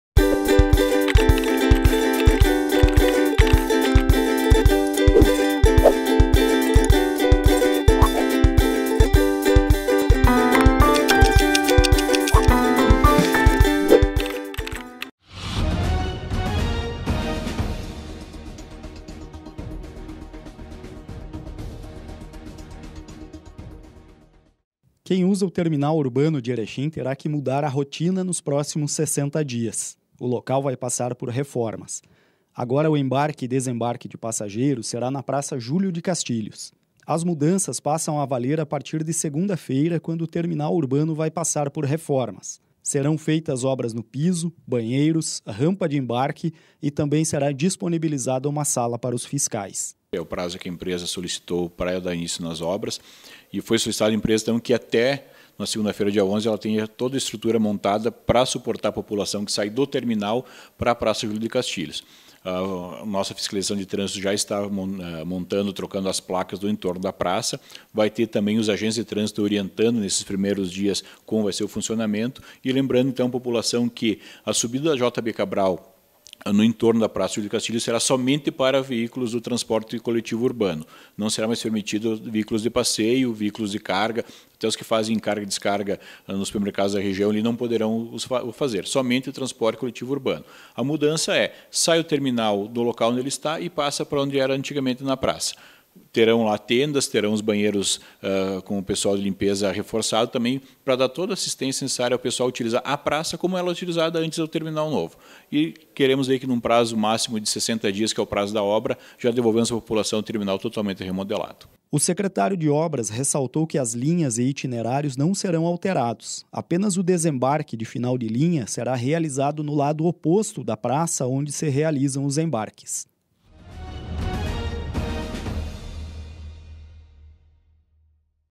REFORMA TERMINAL URBANO
Quem usa o Terminal Urbano de Erechim terá que mudar a rotina nos próximos 60 dias. O local vai passar por reformas.  Agora o embarque e desembarque de passageiros será na Praça Julio De Castilhos.